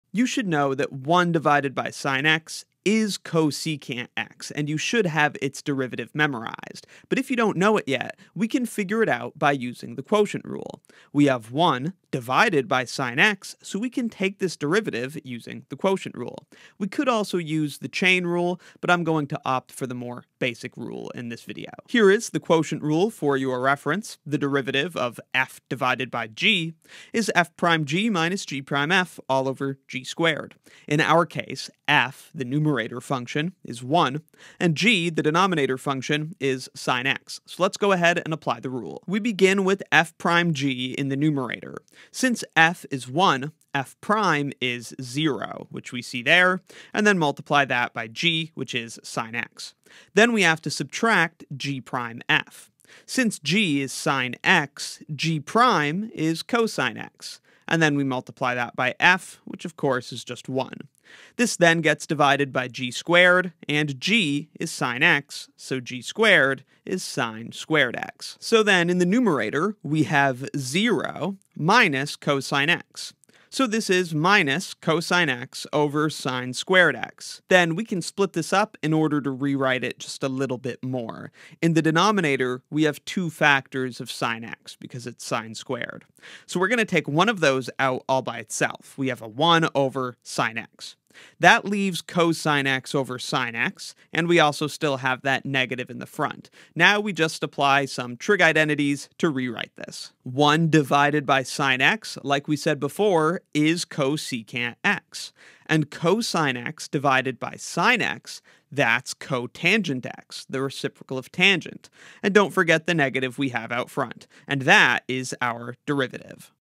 Derivative of 1/sinx (Quotient Rule) | Calculus 1 Exercises
We differentiate 1/sinx using the quotient rule. Note that, by definition, 1/sinx is cscx, so in finding the derivative of 1/sinx we are finding the derivative of cscx. The derivative of cscx is -cscxcotx.